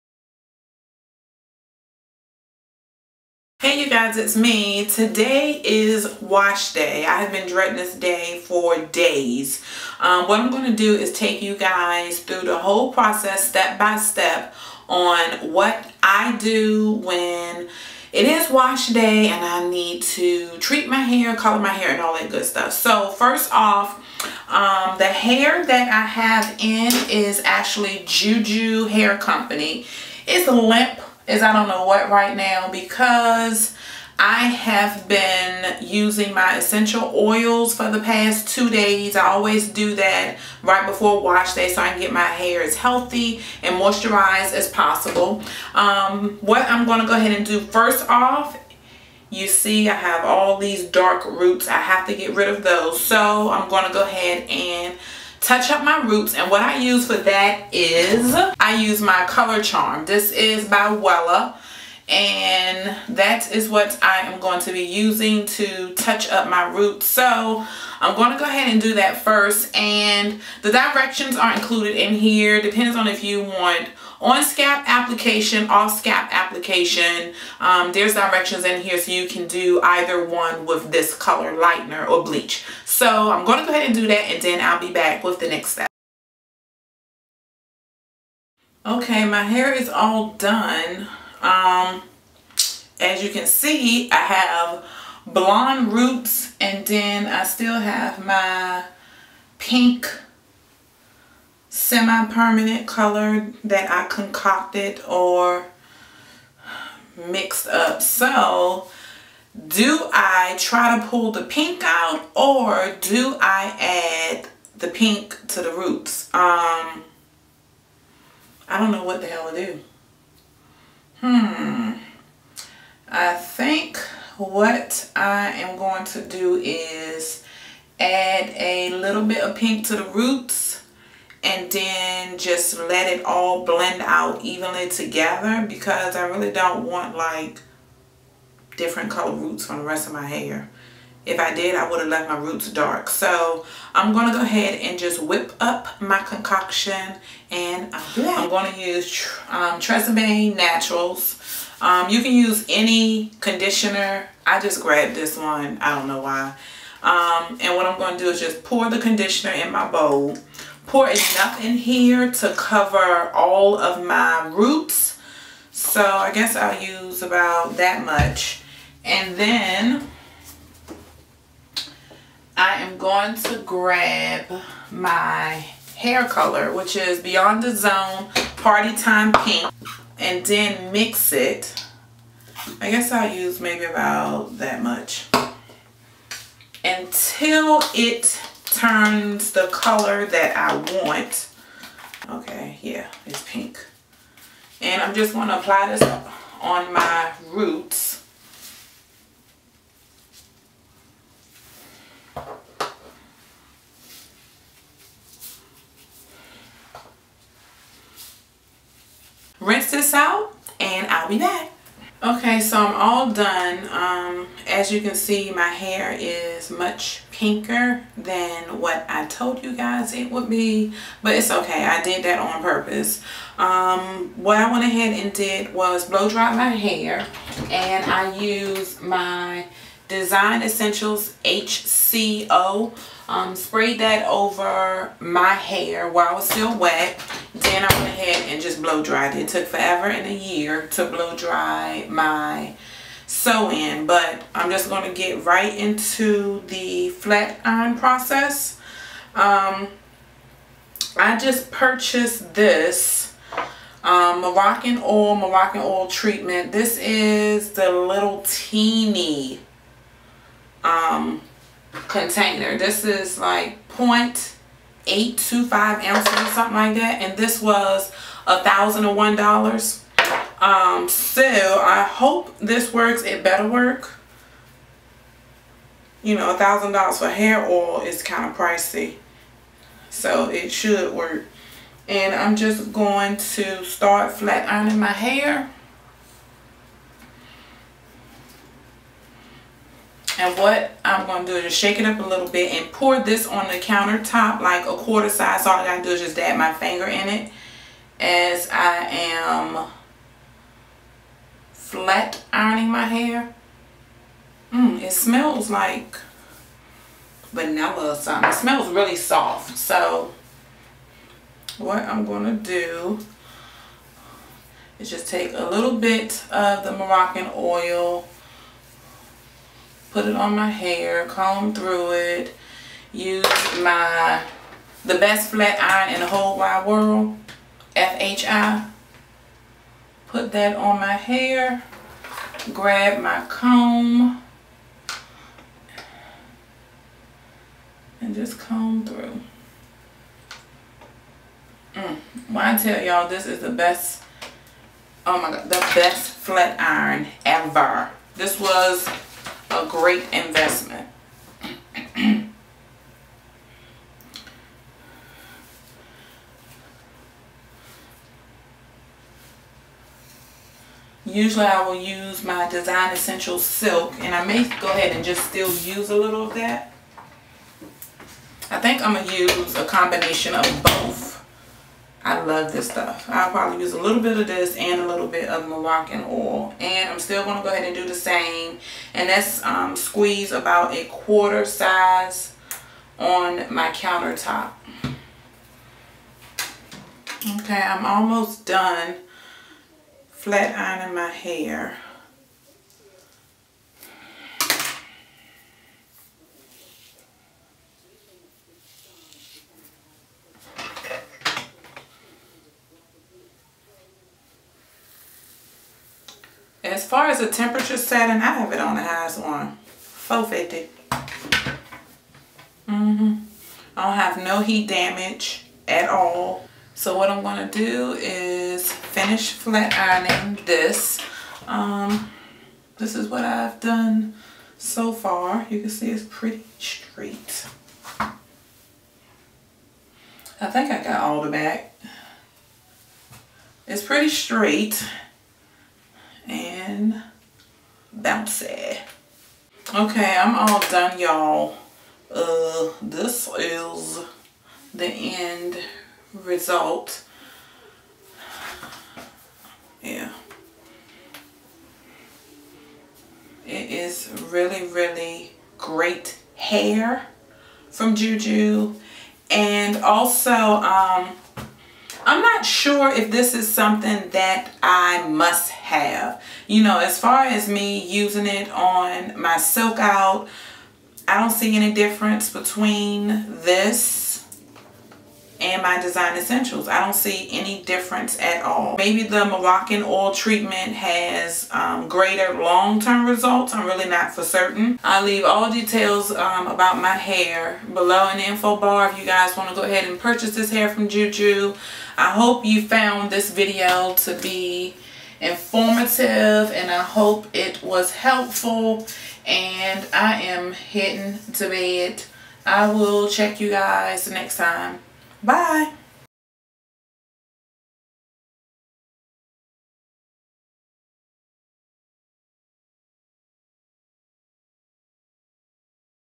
Natural Hair & Weave Maintenance|Typical Wash & Color Day
Watch in HD & EXPAND...

Everything is mentioned is in the video unless otherwise stated below

☆ My Hair ☆
CASHMERE BODYWAVE by JuJu Hair Company

1 Bundle - 24"
1 Bundle - 18"

Also used a small portion of my GH Hair on top (2nd install)

This video is NOT sponsored and no items were provided to me for review